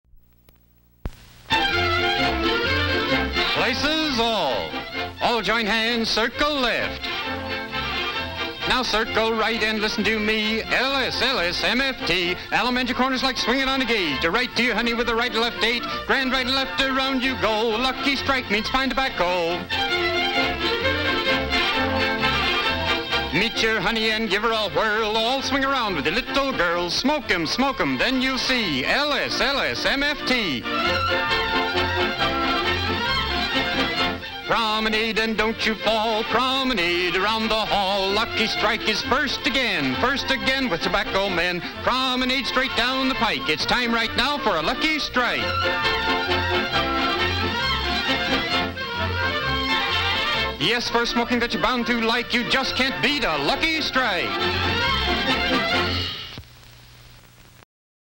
A cigarette square dance
Lucky Strike cigarette commercial. In the days when they were allowed to advertised on TV. Early go at animation.